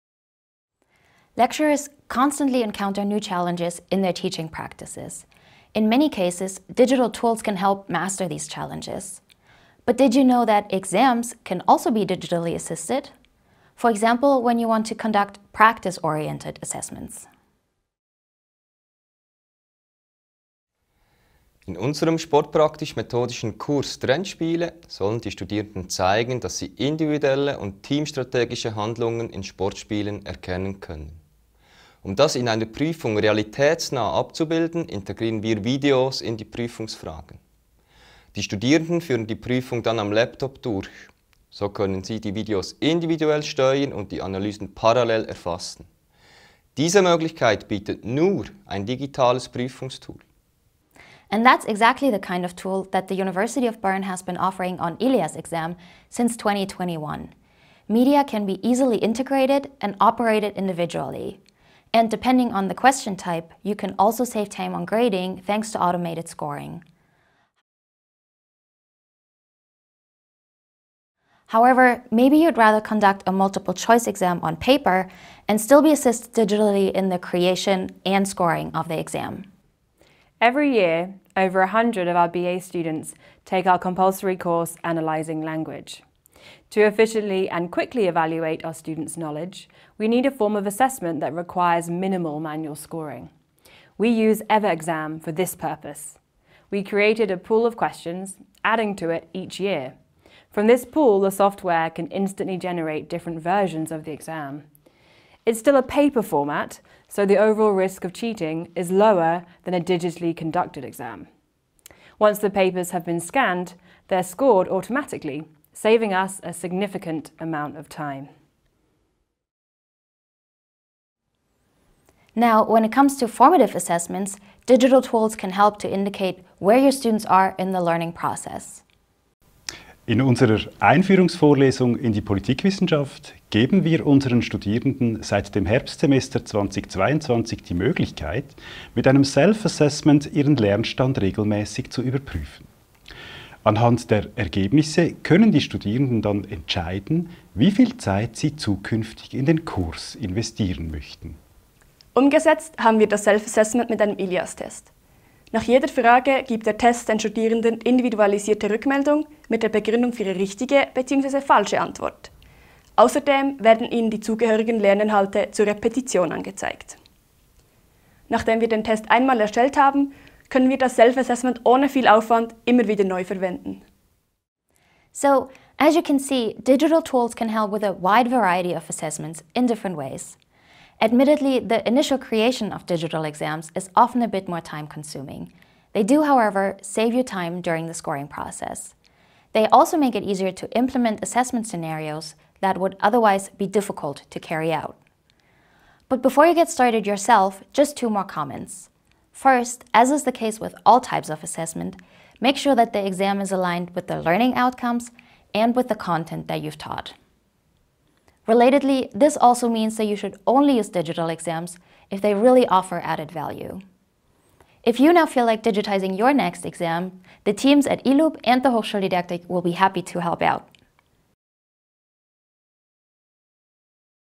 Digital exams – A quick glance at 3 scenarios
Are you familiar with the various options for creating, conducting, and scoring exams digitally? In this video, we guide you through a selection of digital exam options available to lecturers at the University of Bern. Lecturers from various disciplines present very different examination scenarios and explain how certain tools have enabled their implementation. You are sure to find a suitable solution for your needs.

with any questions you may have about digital exams.